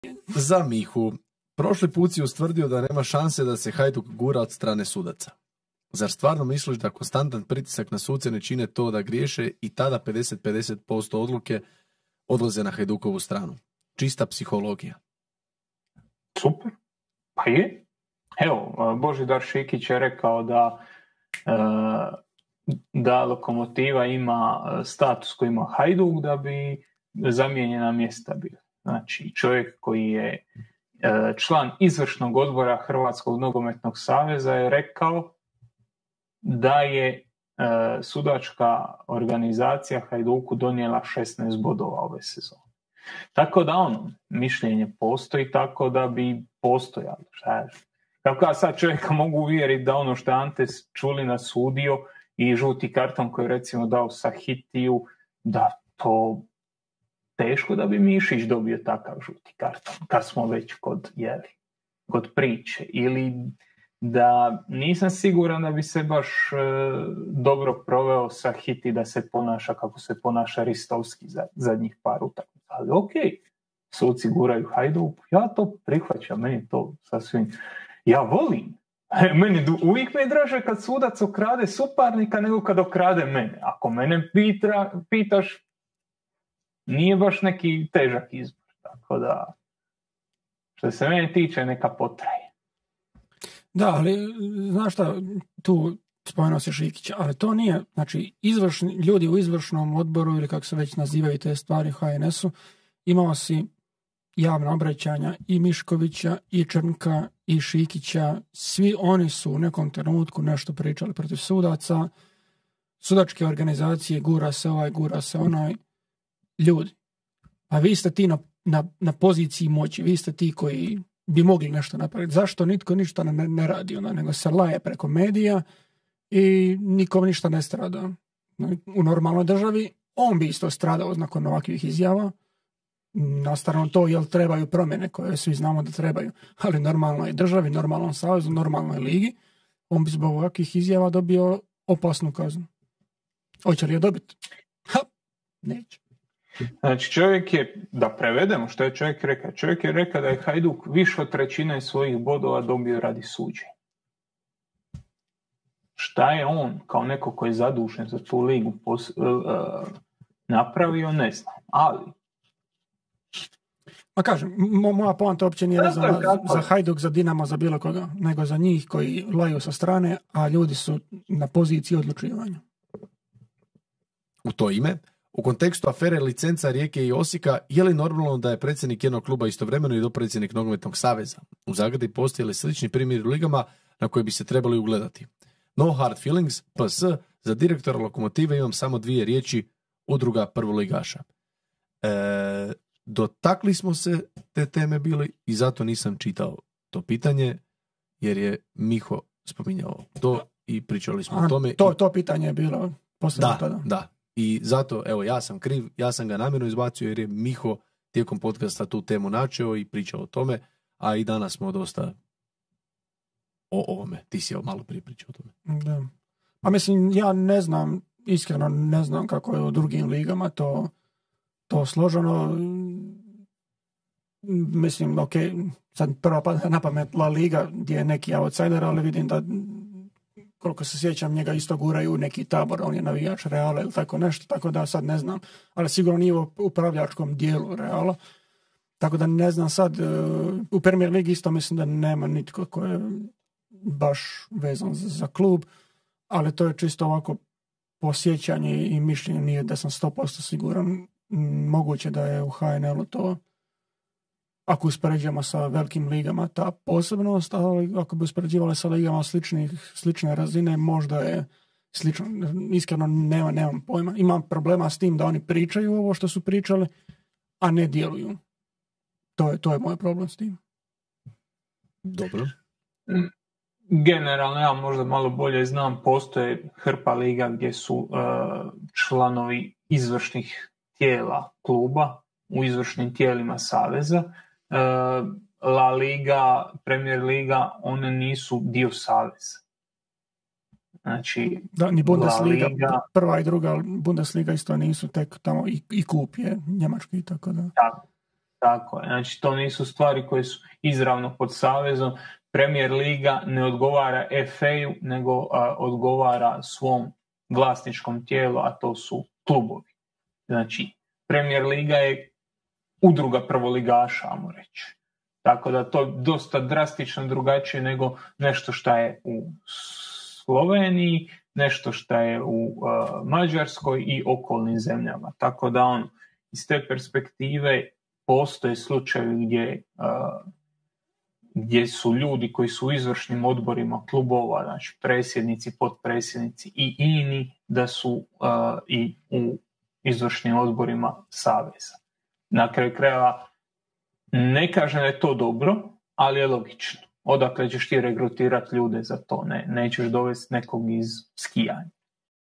"Da imamo suđenje kao Hajduk zamijenili bismo mjesta" - komentar na izjavu B. Šikića
Glavni čovjek Lokomotive je dodatno podgrijao ionako usijanu atmosferu posljednjih mjeseci koja kruži našom ligom, u ovom videu smo se dotakli te izjave i njegova rada.